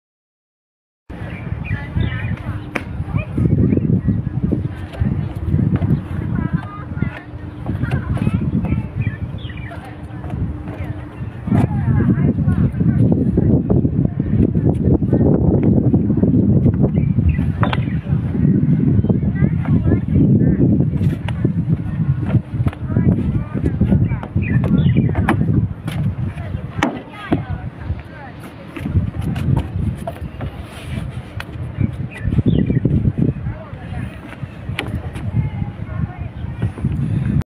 少林羅漢螳螂拳(通臂螳螂拳) 楊老師(お名前不明) 溜腿架 2023年
河北省滄州市滄縣風化店郷大白頭村に傳わる通臂螳螂拳(少林羅漢螳螂拳)です。
楊老師は楊汝立老師のご息女となられます。

原題：「溜腿架」

Lineage；
閻介休(萬修)－恩壽－楊俊普－楊進元(楊俊普之子)－楊德符－楊積善(楊蔭軒、楊八楞)－楊汝立(汝利)